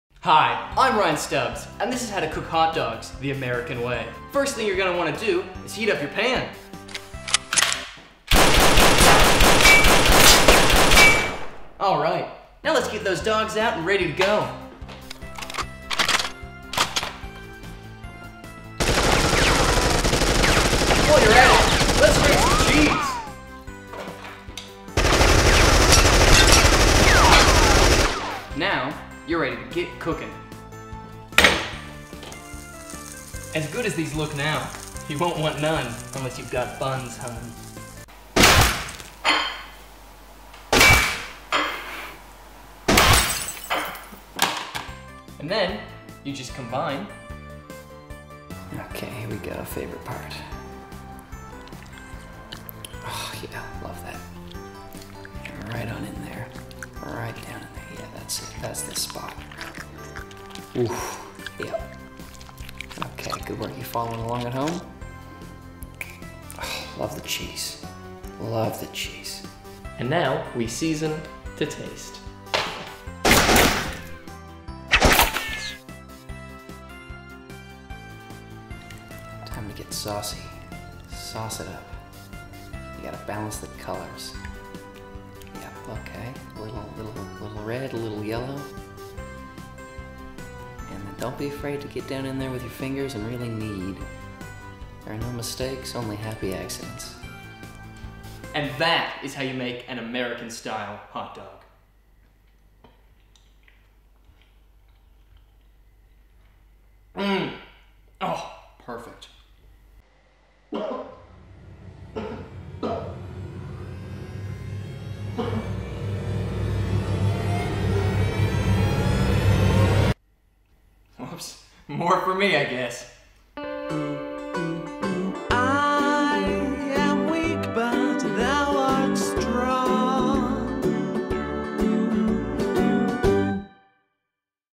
Cooking with Guns: Hot Dogs
Gun specialist and free American Ryan Stubbs takes you through the basics of cooking hot dogs with everyday household guns.